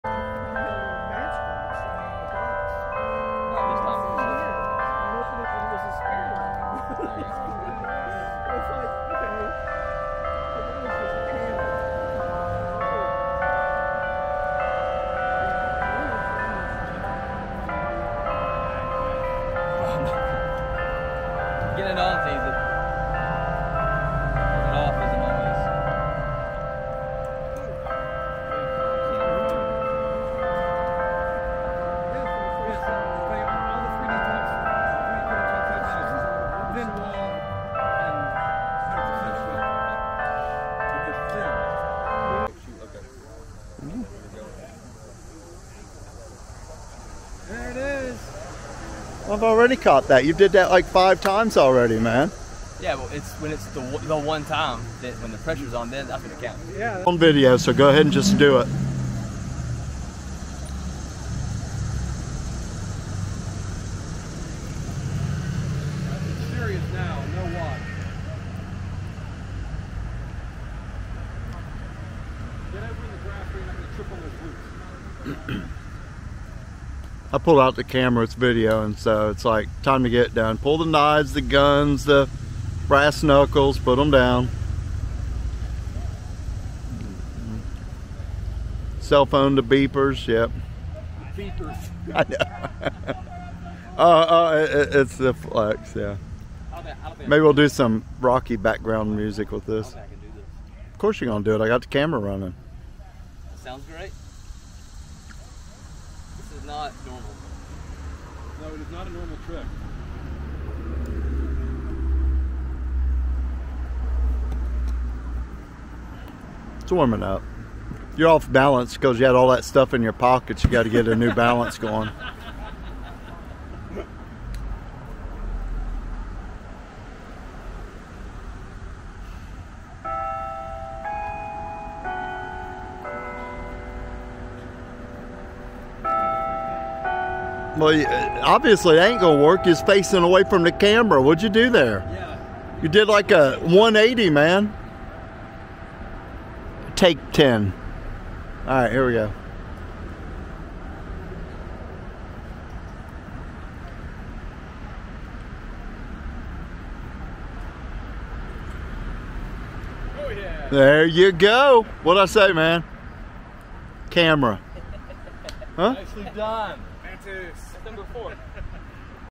Eli H & the Impossible Top Trick - Miamisburg 2024
Eli H shows some skill with the Tops at the Miamisburg Tops Festival in Aug 2024.  Watch till the end for the impossible trick :)  Many talented people show up every year...